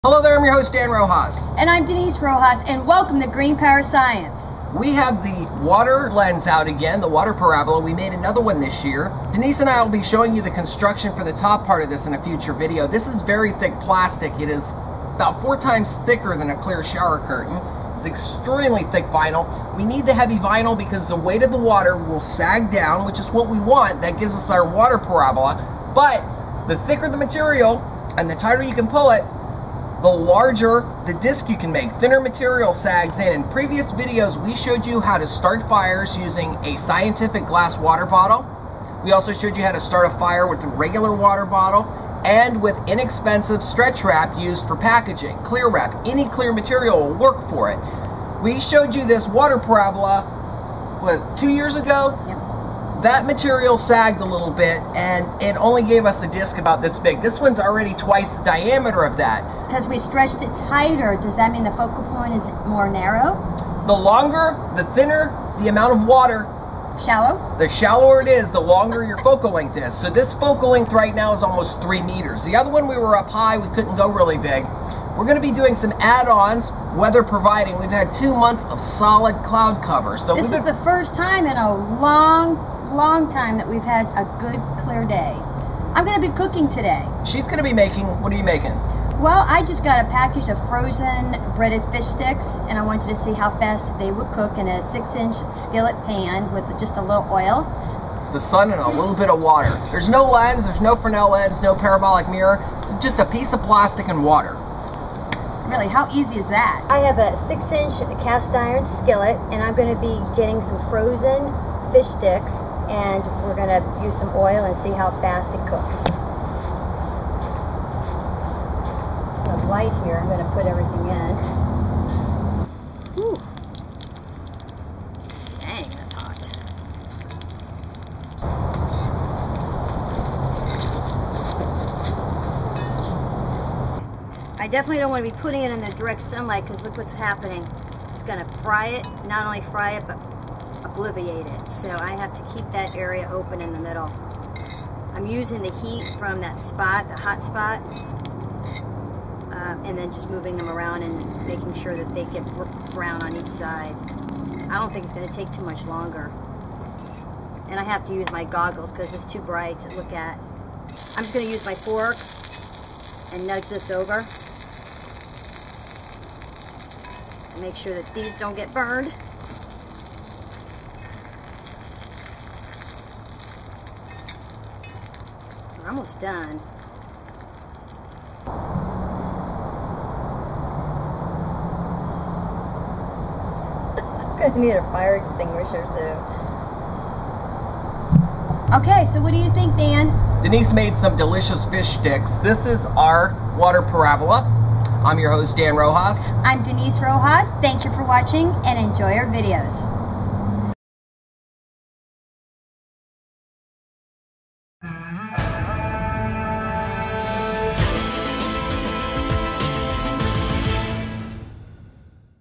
Solar Death Ray Cooking with a Water Lens
Solar cooking is easy in the summer when the sun is directly overhead. This plastic vinyl sheet has a focal length exceeding 3000mm and produces temperature in excess of 1400˚ F or 760˚ C.